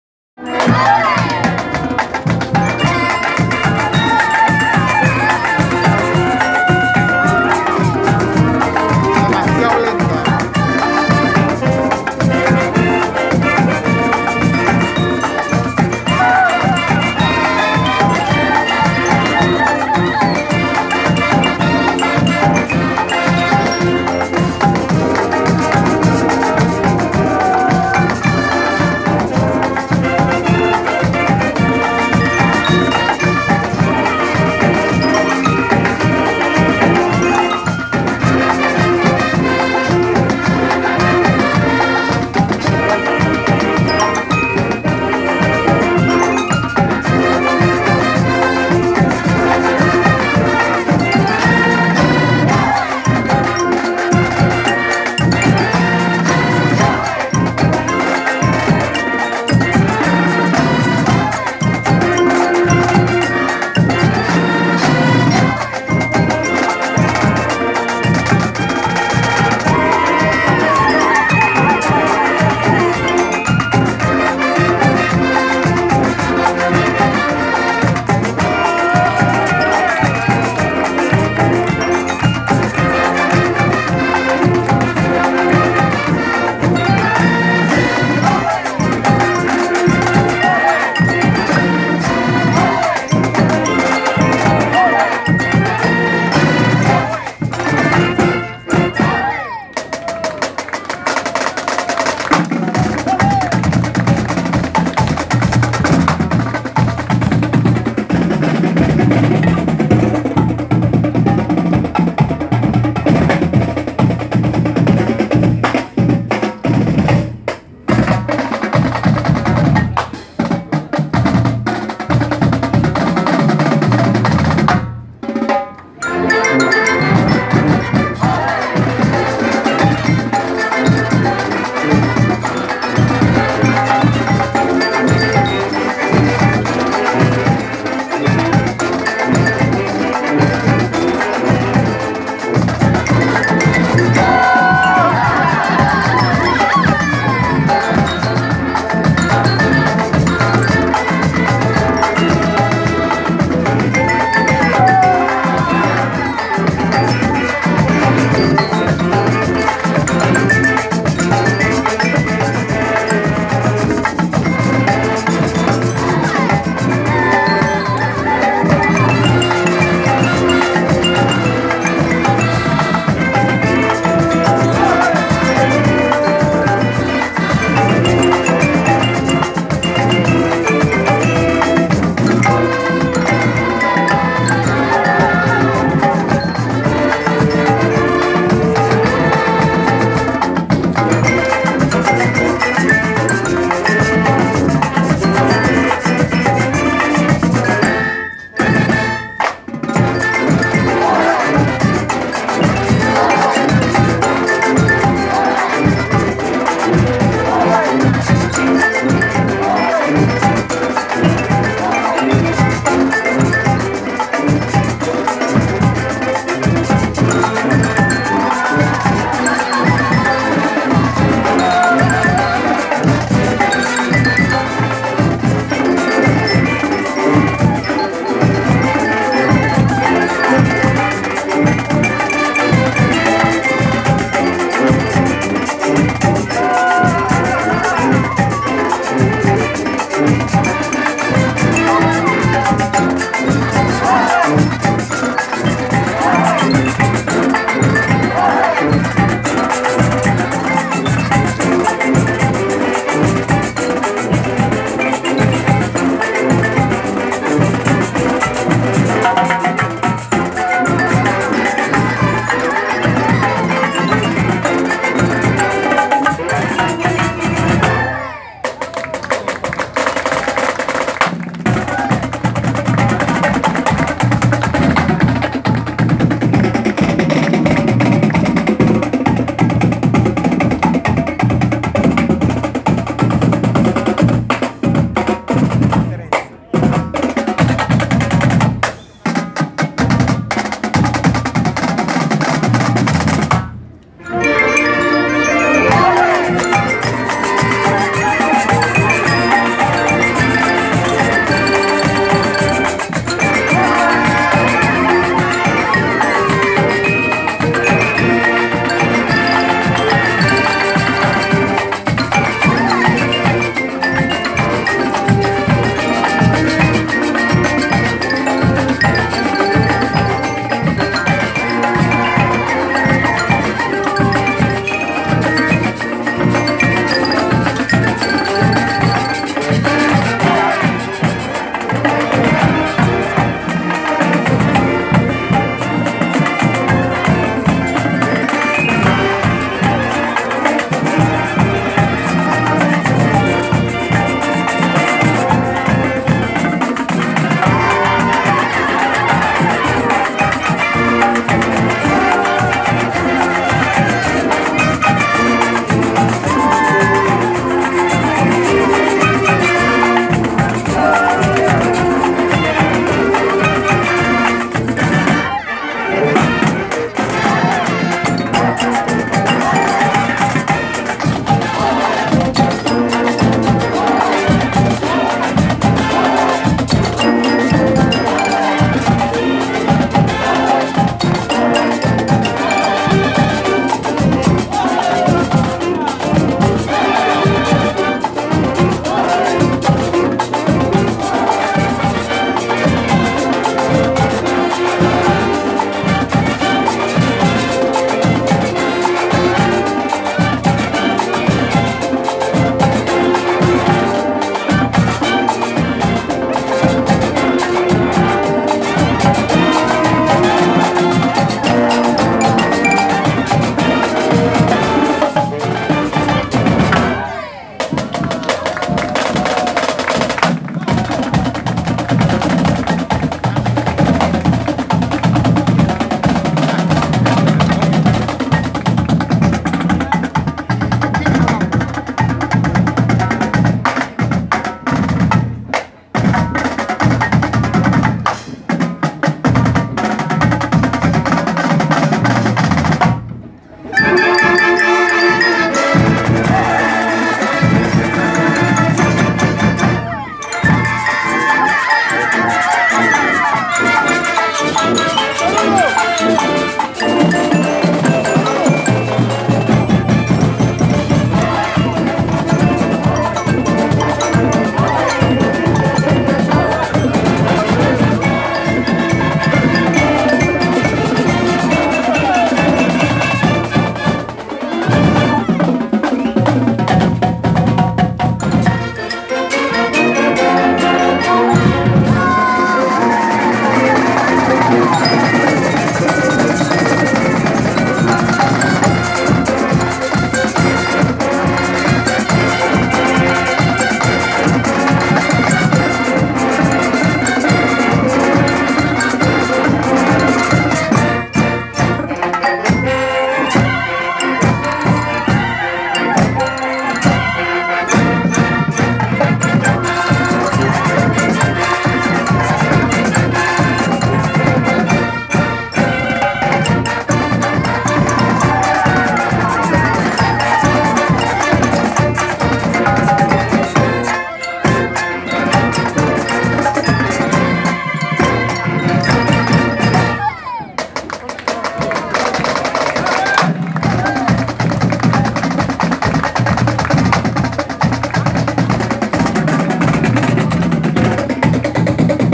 Ballet Folklórico Banda Colegio MMTR LAS TABLAS
Desfile Fundación La Pintada - Coreografías y Montajes: EDGAR ALEXIS VILLAVERDE 2017.